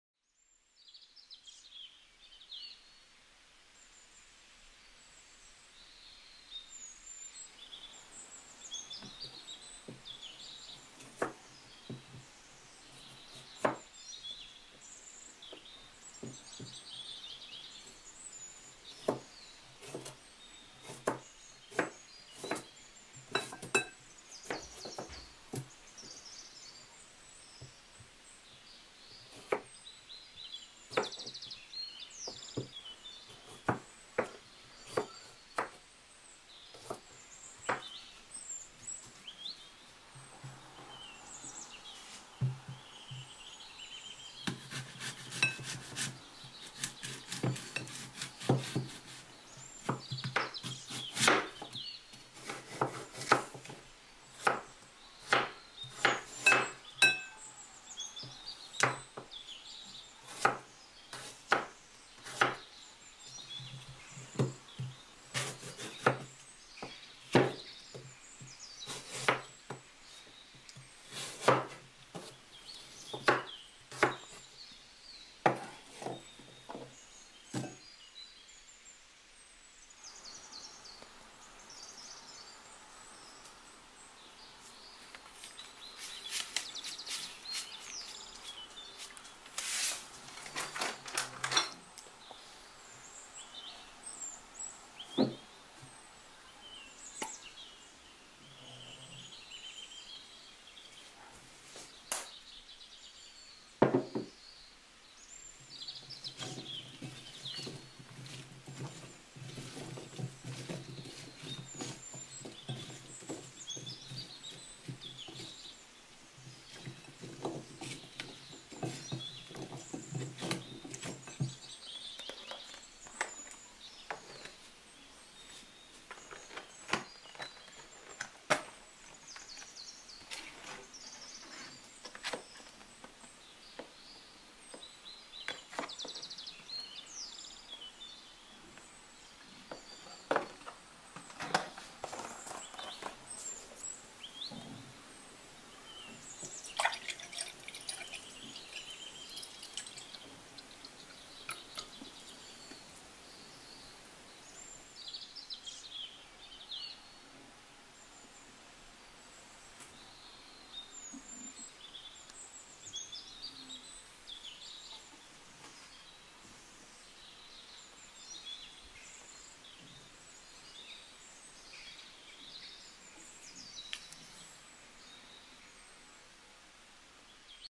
My Steam Bun Breakfast With Salad And Apple Juice
Today I'm having vegetable steam for my breakfast. Also have added cucumber and apple salad mixed with black pepper and sesame oil. I am also having a glass of apple juice. Hope you enjoy.